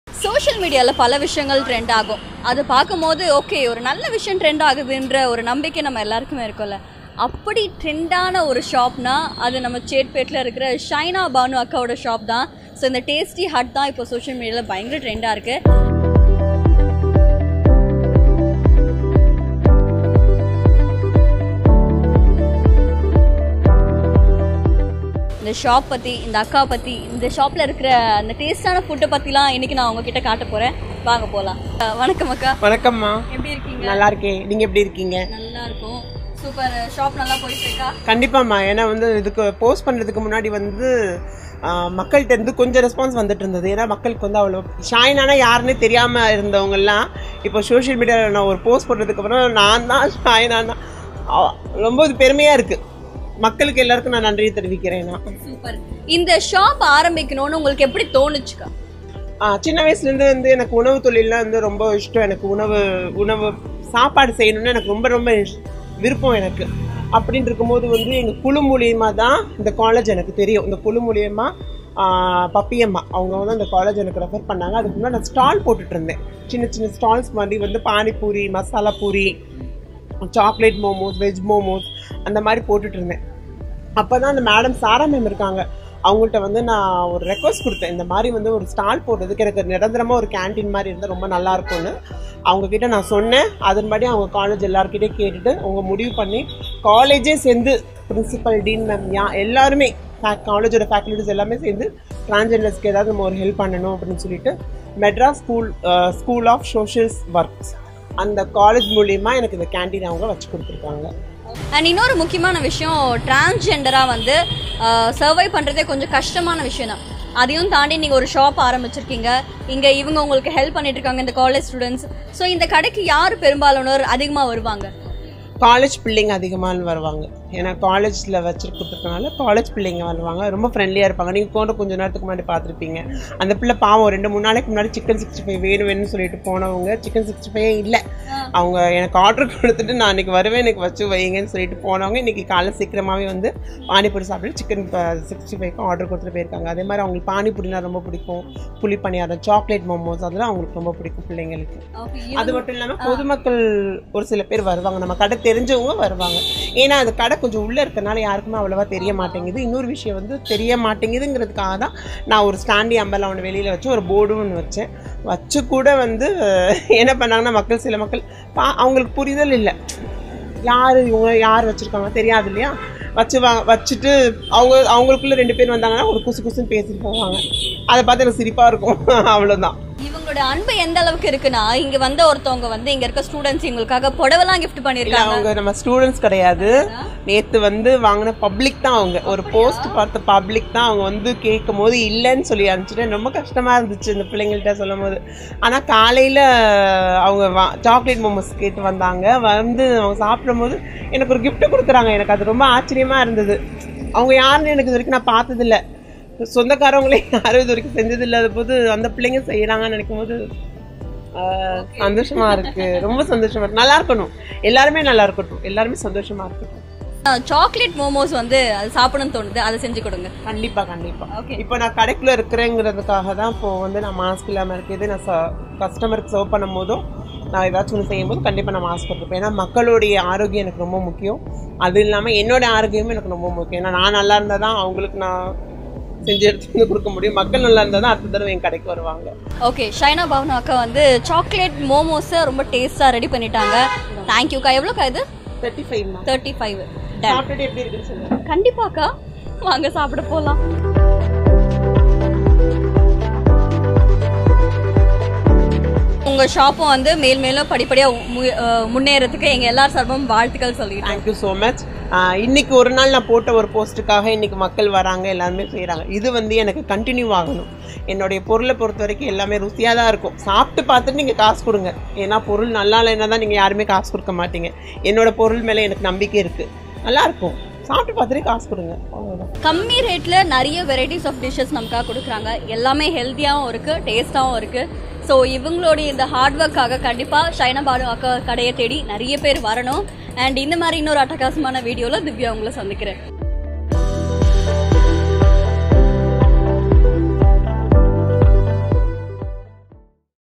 சென்னையில் திருநங்கை நடத்தும் உணவகம்....
Shaina Banu - Transgender Tasty Hut, MSSSW College, Egmore, Chennai...